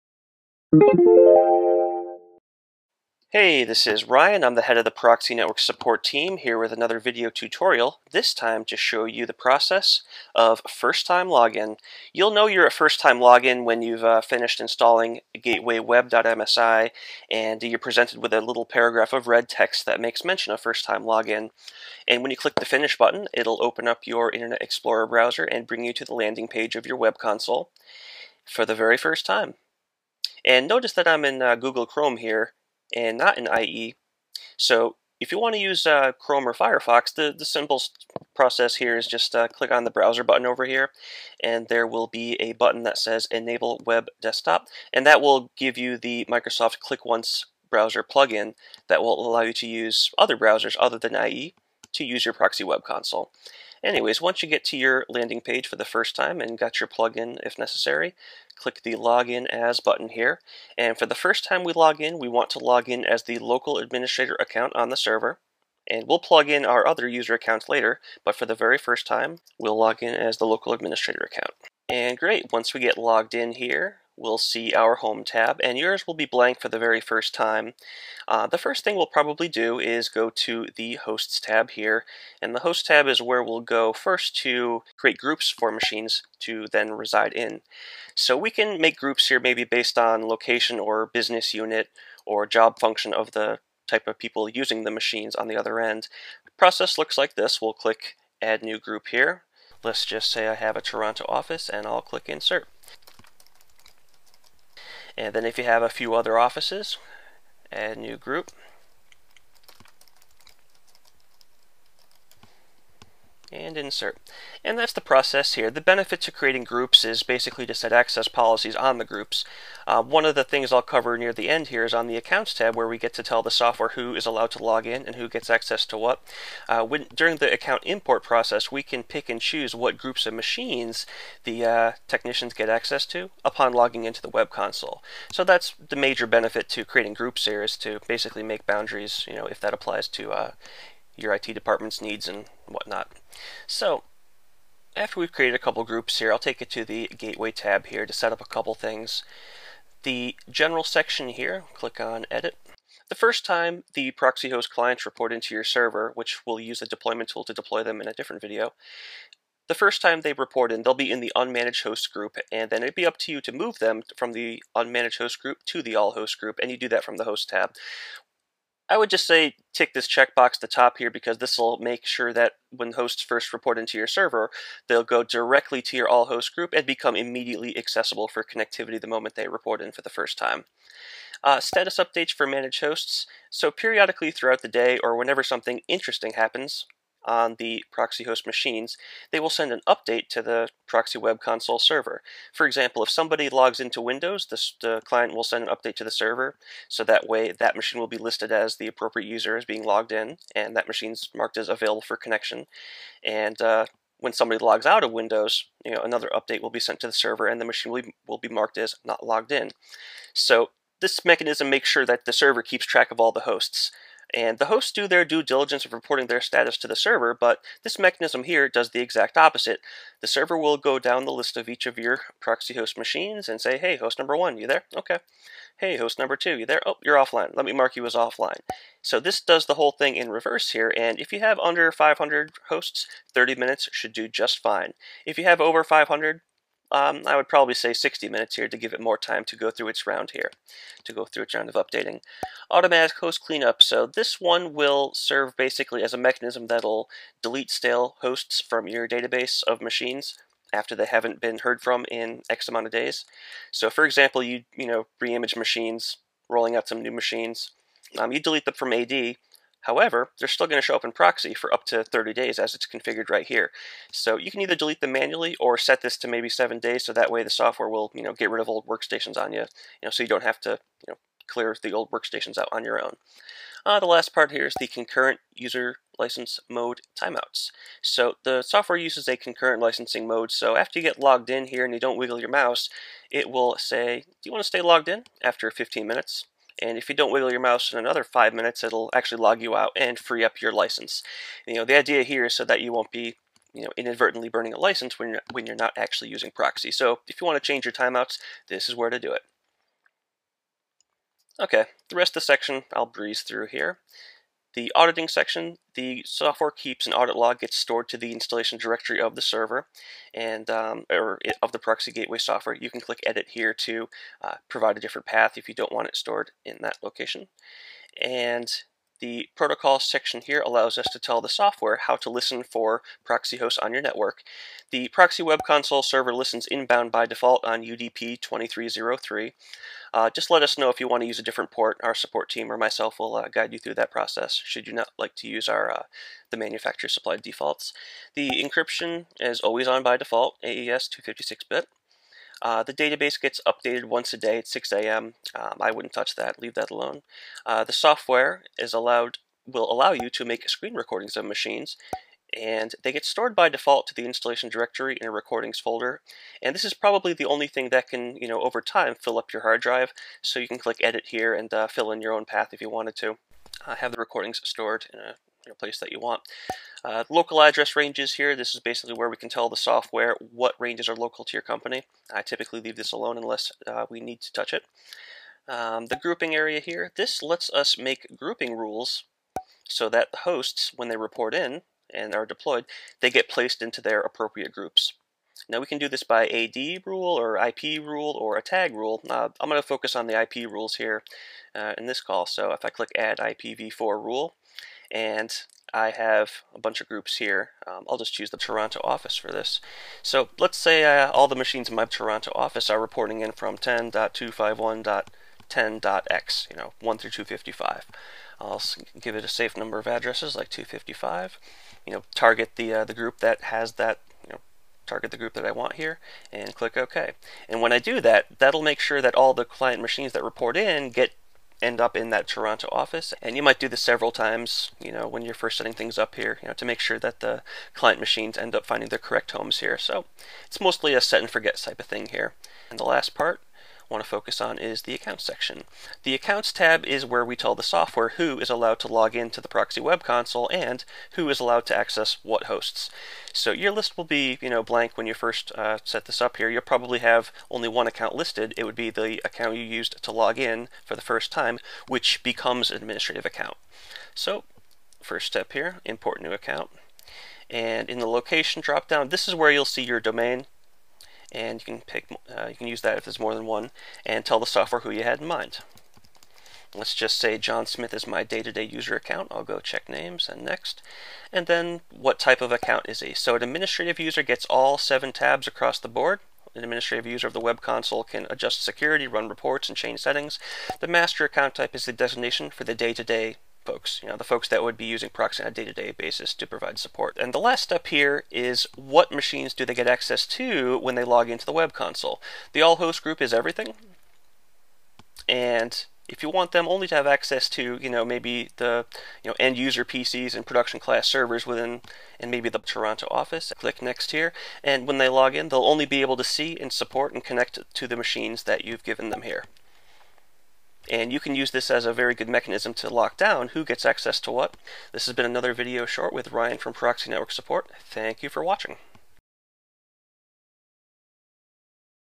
PROXY Pro Private Cloud Edition - First Time Login/Setup Walkthrough
to see how our remote access solutions can revolutionize your IT department today.

This video starts where the Proxy Web Console Installation Guide PDF gets you to the point of "First Time Login", which is after you've installed GatewayWeb.msi.  Watch to learn best practices according to the Proxy Networks Support Team!